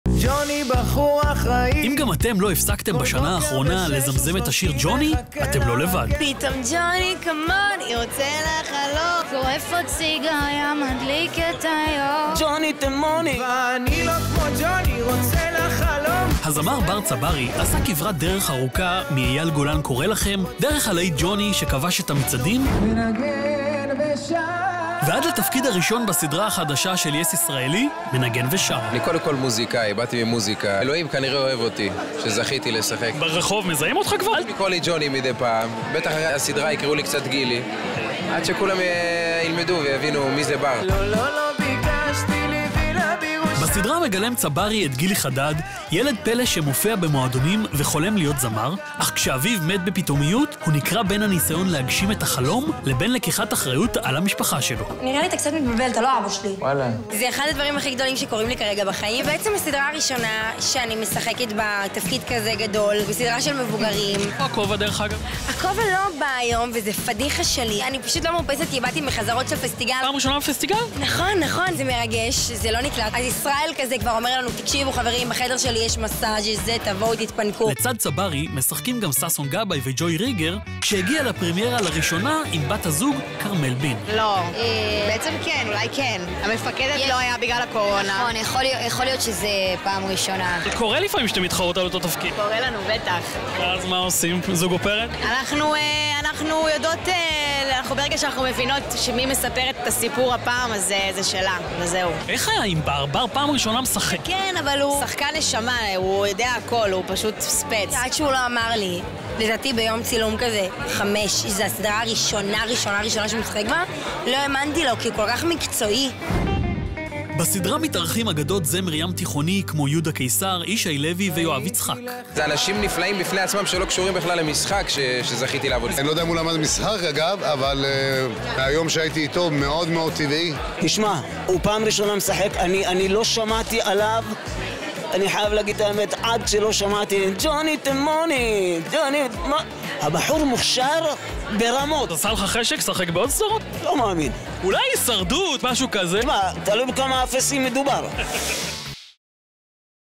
בר צברי מגיע לטלוויזיה: "אלוהים כנראה אוהב אותי"
קפצנו להשקת סדרת הדרמה החדשה "מנגן ושר", שתשודר מחמישי הקרוב בערוץ yes ישראלי, ושוחחנו עם כוכבי הסדרה בר צברי, קים אור אזולאי וג'וי ריגר